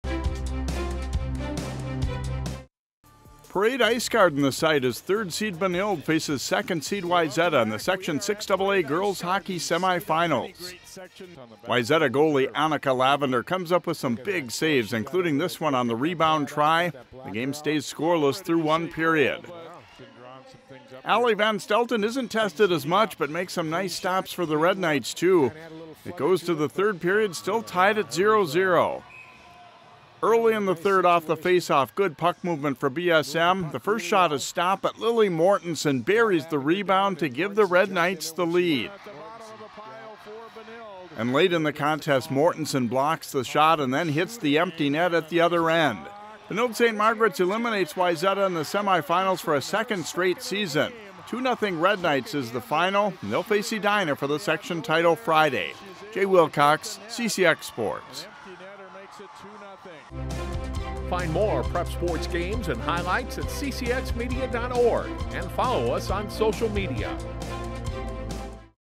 Benilde-St.Margaret’s Girls Hockey Beats Wayzata to Advance to Section 6AA Final
The Benilde-St. Margaret’s girls hockey team edged Wayzata 2-0 in the Section 6AA semifinals Wednesday night at Parade Ice Garden. The Red Knights broke a scoreless tie on a rebound goal by Lily Mortenson early in the third period. Mortenson added an empty net goal to secure the win for the Red Knights. It’s the second straight season that BSM has eliminated Wayzata in the semifinals.

Benilde-St. Margaret’s meets top seed Edina for the section title Friday night at Parade Ice Garden.